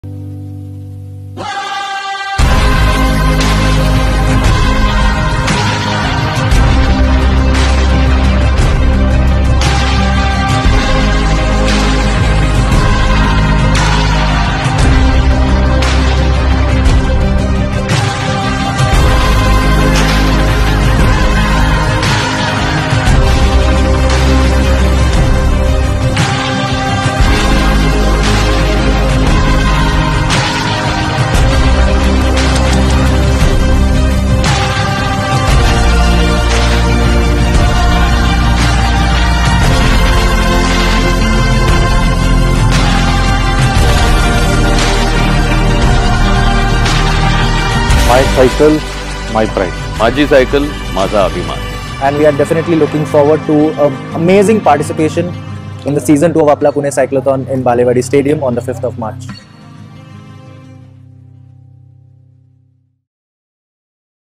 Apla Pune Cyclothon Season 2 Official Trailer | Champ Endurance
The city of Pune rises for a revolution, to bring back Pune’s prestige as a cycling city.
Apla Pune Cyclothon Season 2 on 5th March, 2023

Share a video of you sharing your love for cycling and use the tag

An Event By Champ Endurance